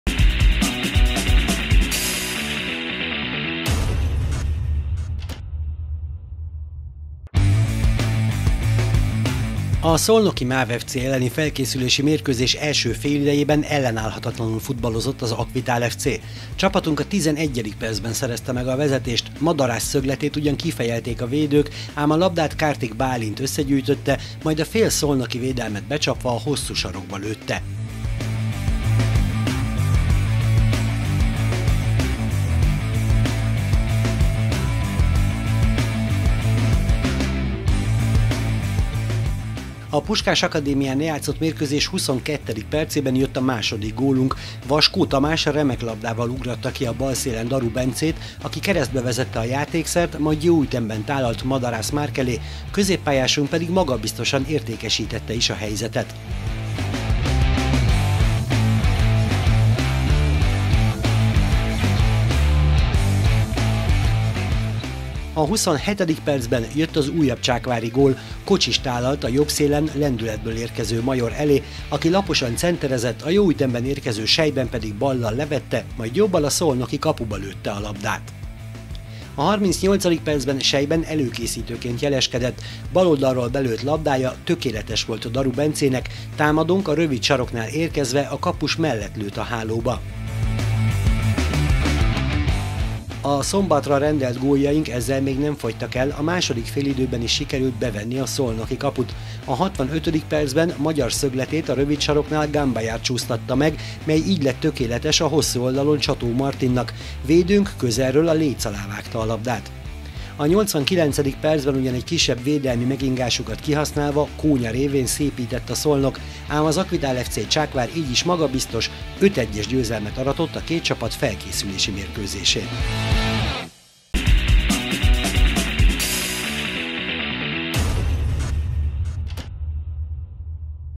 5-1-re nyertünk a Szolnoki MÁV ellen
Már az első félidő után 4-0-ra vezettünk NBII-es ellenfelünk ellen, végül magabiztosan nyertük a felkészülési mérkőzést